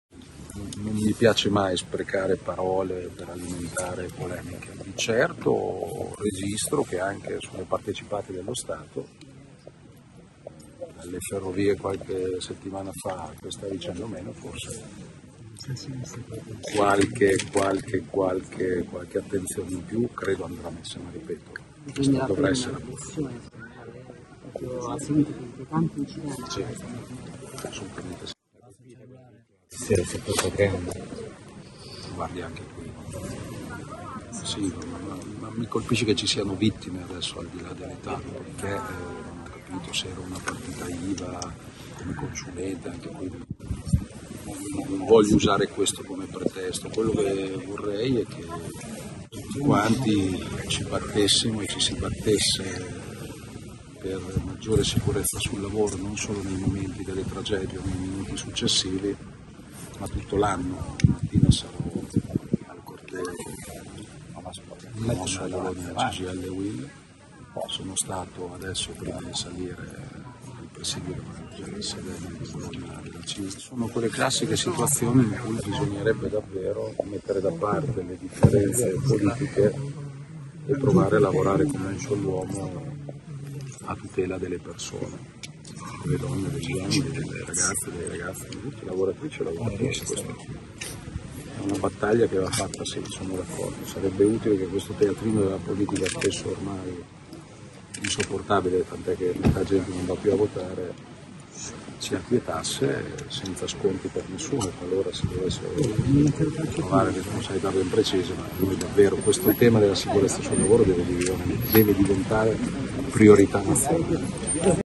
Esplosione Suviana, Bonaccini: su sicurezza no a colori politici
Su partecipate statali andrà messa qualche attenzione in più